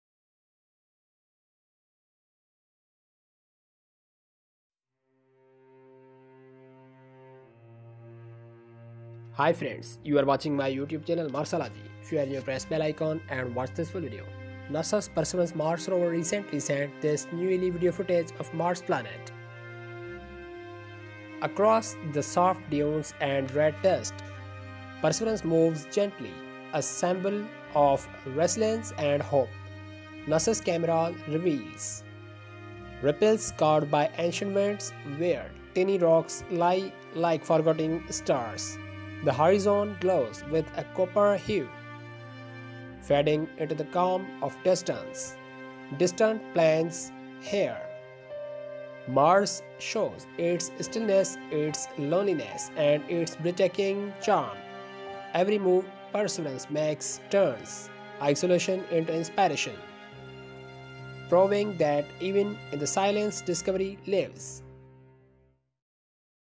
NASA Mars Rover Newly Unviel Mars Video Will Blow Your Mind | Shocking 4k Footages of Mars Sol 1912
Credit: NASA.GOV
NASA/JPL-Caltech

NASA's Mars Rover Perseverance has sent the most incredible footage from Mars, capturing stunning Mars images and breathtaking landscapes of the Red Planet. This latest Perseverance Rover video showcases NASA’s groundbreaking exploration, revealing never-before-seen details of Mars’ surface. As Mars Rover Perseverance continues its mission, it uncovers new secrets of the Martian terrain. Watch this exclusive Mars latest video and witness NASA’s cutting-edge technology exploring the mysteries of Mars.